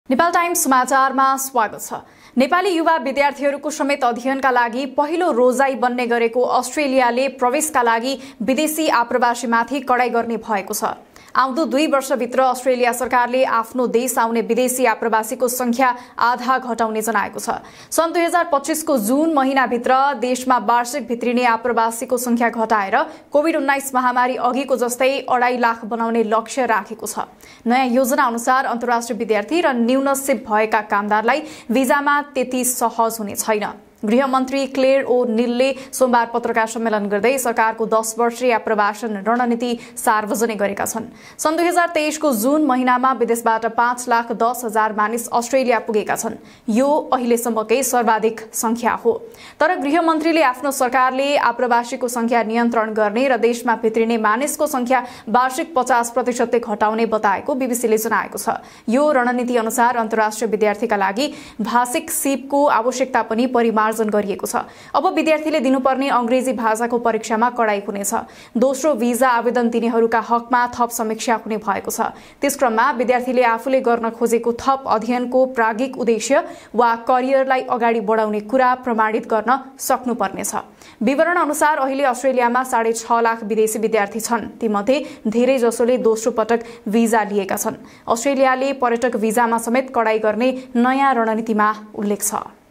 अस्ट्रेलिया जान विद्यार्थी, कामदार र पर्यटक सबैलाई कडाइँ || Nepal Times
अस्ट्रेलिया जान विद्यार्थी, कामदार र पर्यटक सबैलाई कडाइँ

Produced By: Nepal Times Media Private Limited
News Edited By: Harihar Nepal
Presented By: Rashmita Udash
Video Edited By : Nepal Times